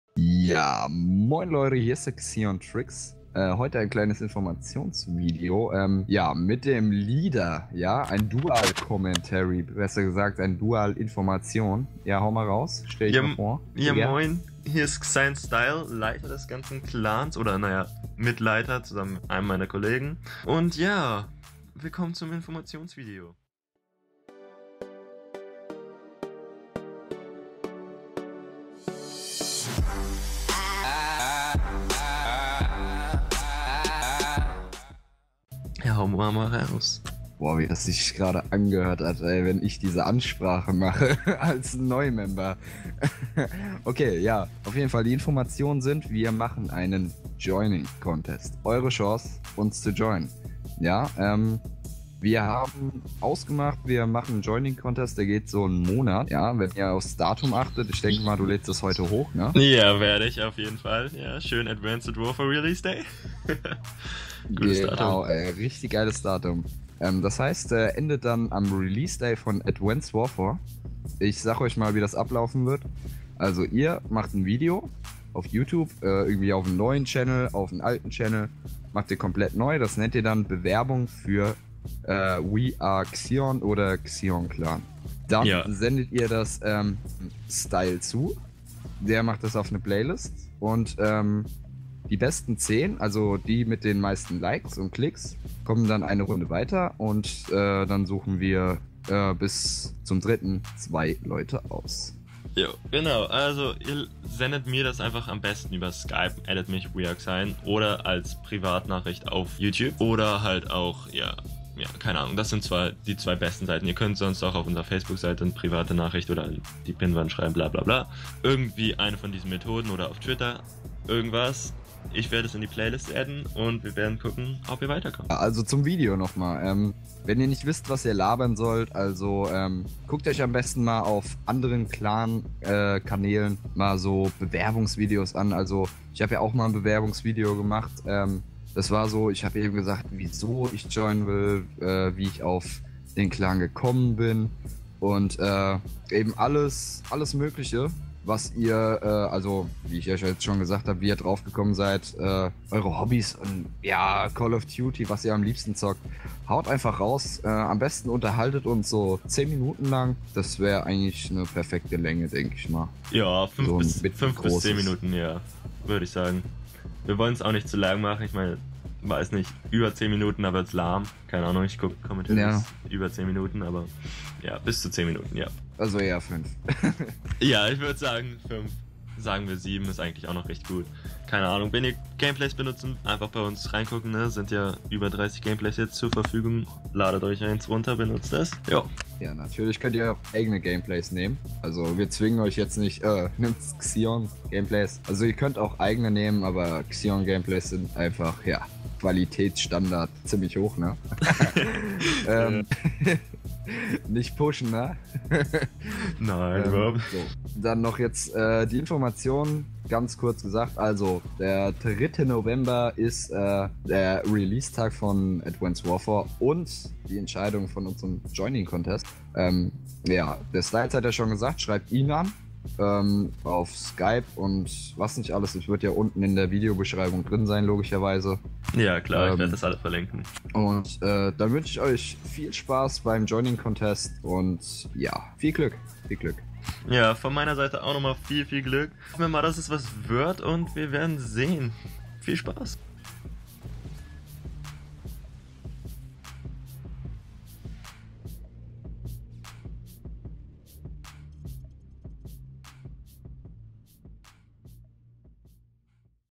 XiEoN Clan Recruitment Challenge | CLOSED
►Willkommen zu unserem JOINING CONTEST !
►Mach mit und mit und werde ein Teil des XiEoN Clans!!
►CONTEST IS CLOSED
                                         ★Infos★
►Postet euer Video in die Kommentare oder meldet euch bei XiEoN_Style !

► XiEoN_Style :
➥ Skype : WeAreXiEoN

►Kommentatoren:
✘ XiEoN_Style :
➥ (Leader)

Video Uploaded by WeAreXiEoN (XiEoN Clan/ XiEoN)